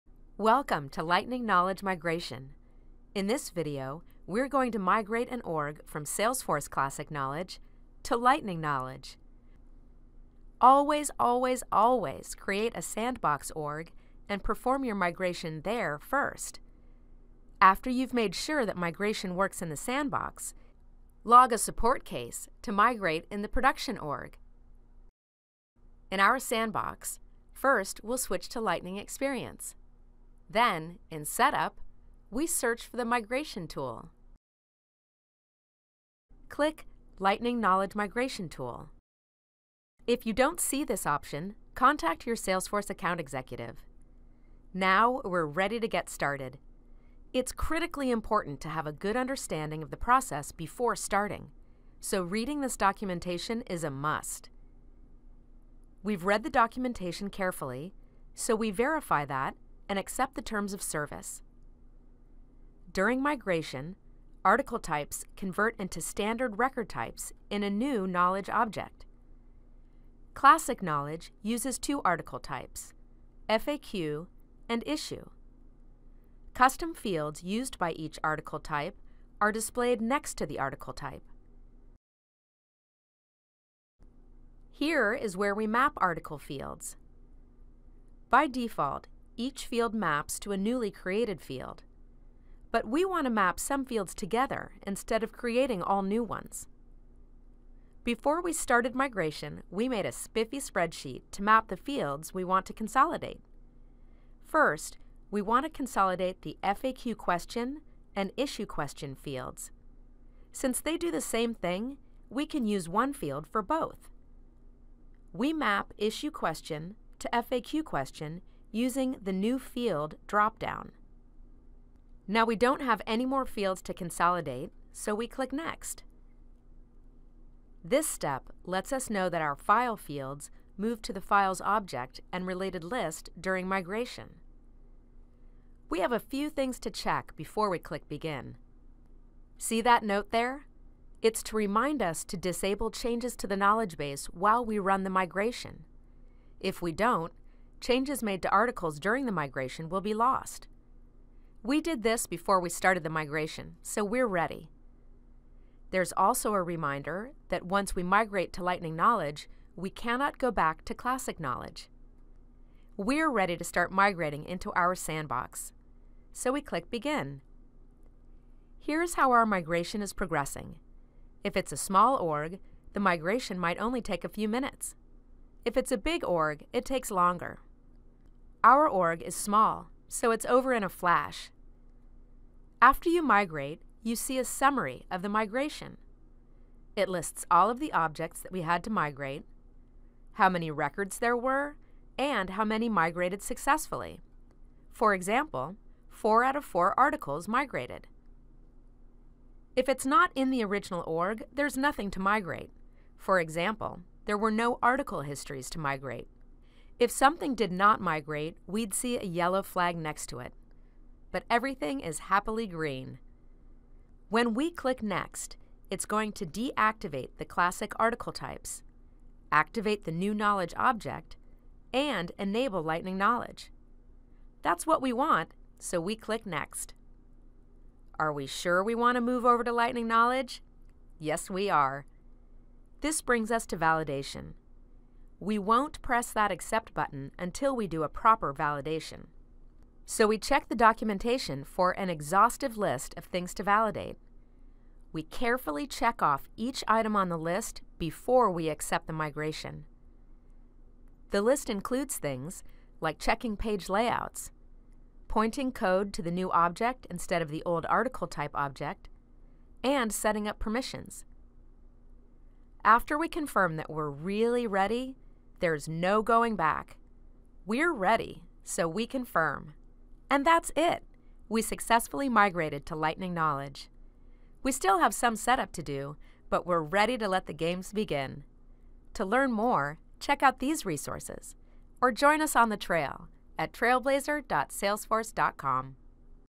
Lightning Knowledge Migration Tool | Salesforce
Watch a sample migration from Salesforce Classic Knowledge to Lightning Knowledge. We show how to map article types to record types, start a migration, validate the migration, and finish migrating the knowledge base. Here’s a link to documentation about the Lightning Knowledge Migration Tool